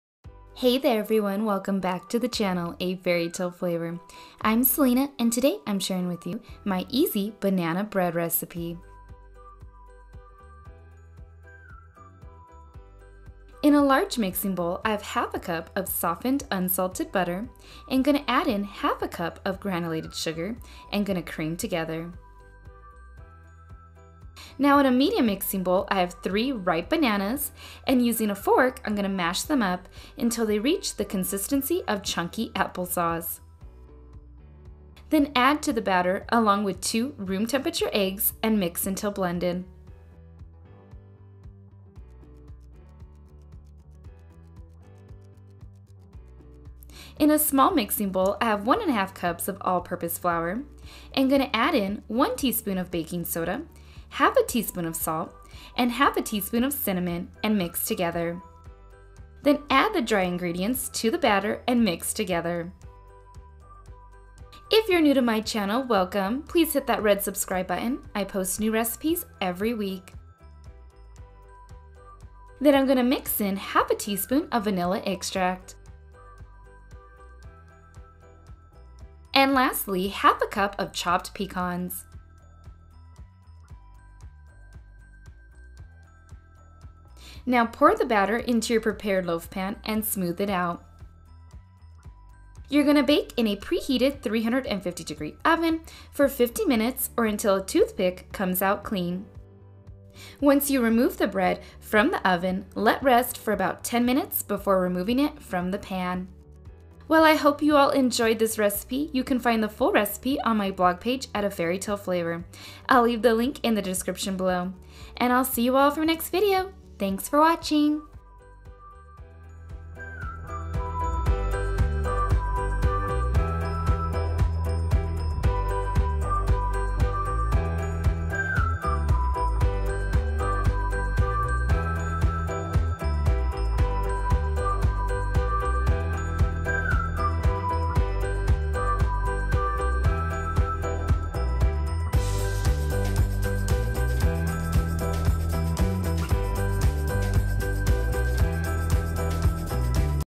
HOW TO MAKE THE BEST BANANA BREAD | MOIST BANANA BREAD RECIPE
This moist Banana Nut Bread with Pecans is easy to make, tastes amazing, and will be your families new favorite banana bread recipe!